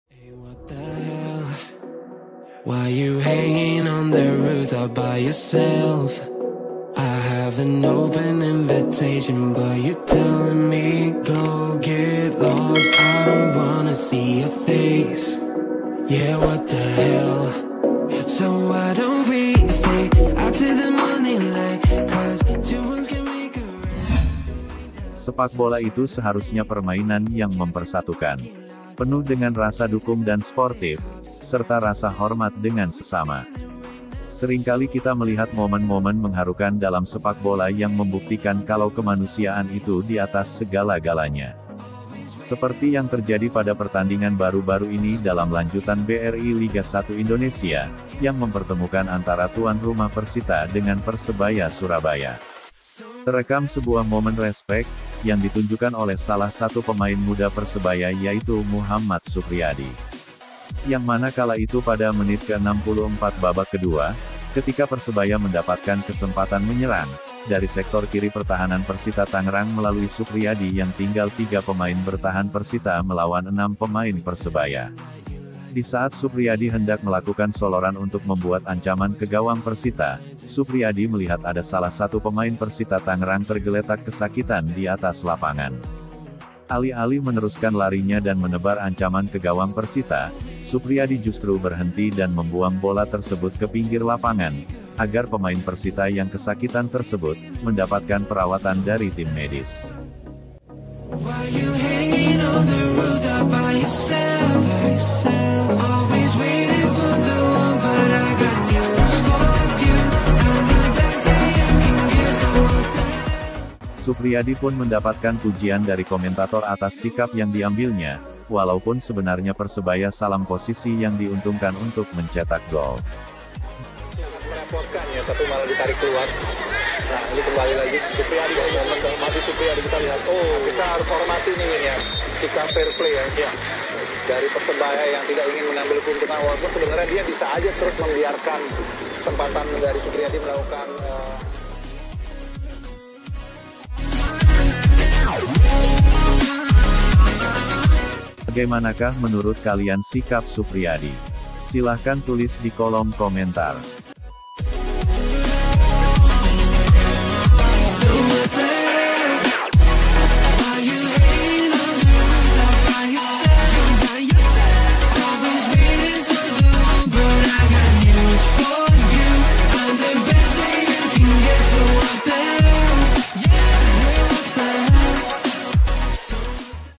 "BESARNYA HATI SANG PEMAIN MUDA" Lihat bagaimana Moment Respect yang Dilakukan Pemain Muda Ini
aksi respect yabg dilakukan supriadi saat pertandingan persita vs persebaya liga 1 Indonesia